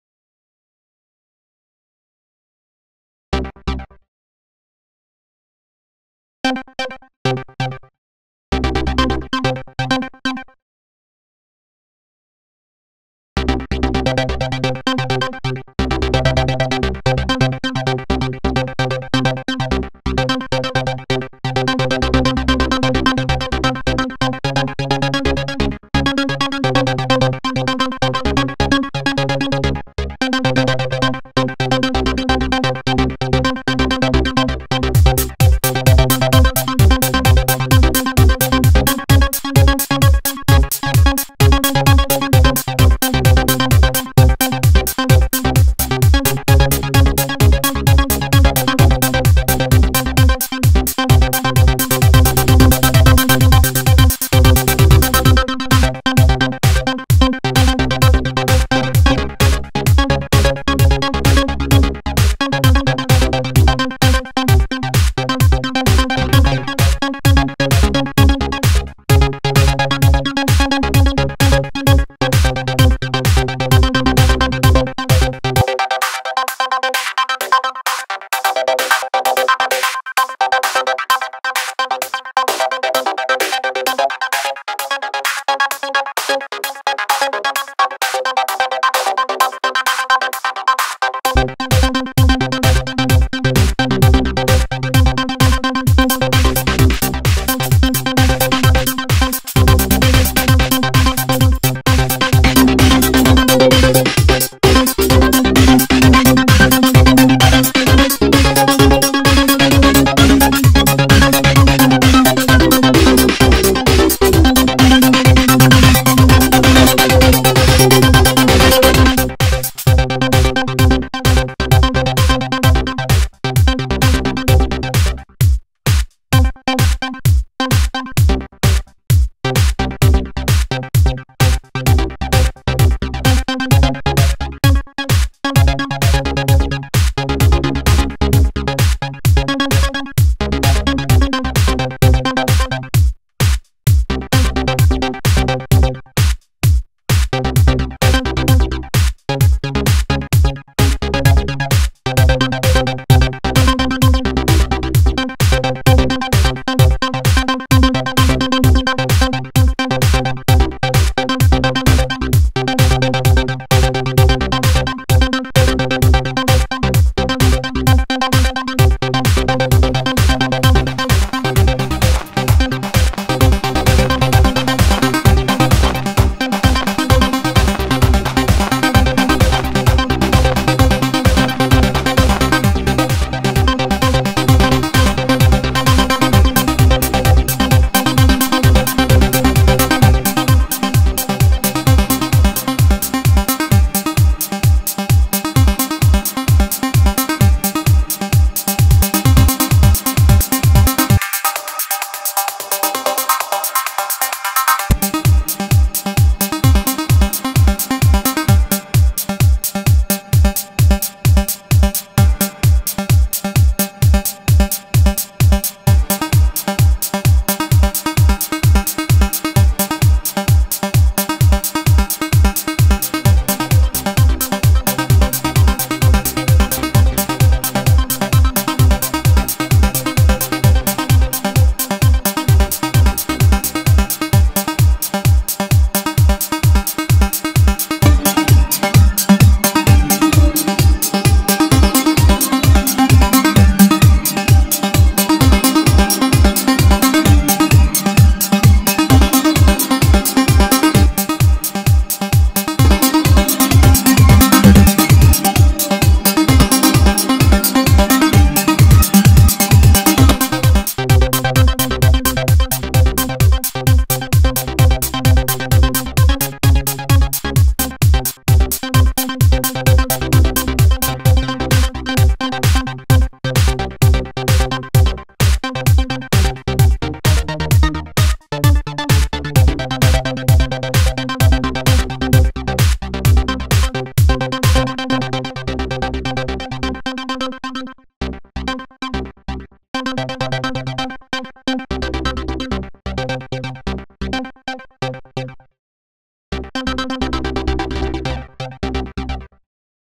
Jazzmutant Lemur
Day 3 with my Jazzmutant lemur. I am using Reason 4 and Ableton Live. I modified the Bumbalizer template and I am using a custom mixer with clip manipulator